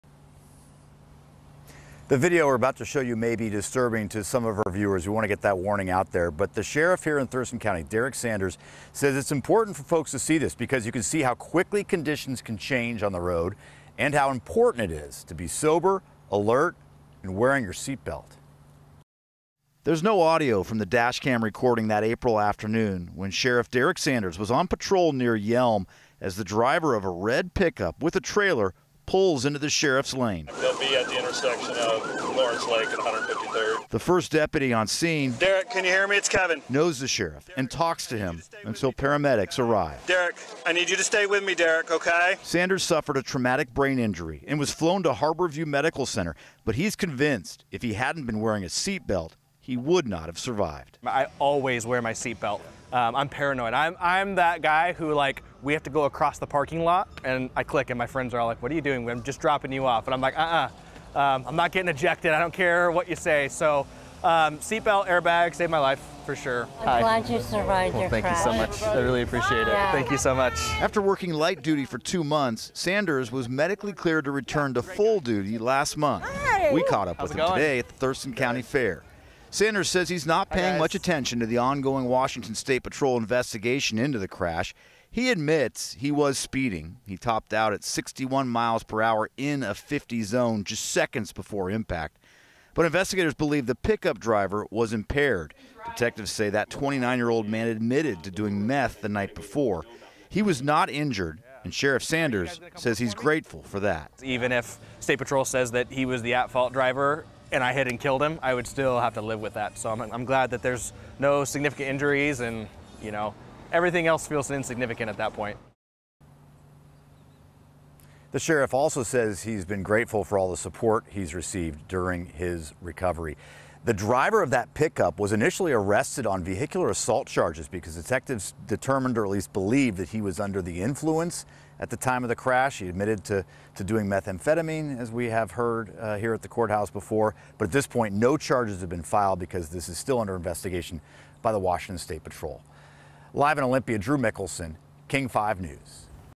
Thurston County Sheriff says seatbelt, airbag ‘saved my life’
Thurston County Sheriff Derek Sanders, back at work after suffering a traumatic brain injury in an April car crash, said wearing a seatbelt saved his life.

“For sure,” said Sanders, “I always wear my seatbelt.”

On the afternoon of April 2, south of Yelm, Sanders was driving his Thurston County Sheriff’s Office vehicle when someone pulled out in front of him on Lake Lawrence Road Southeast.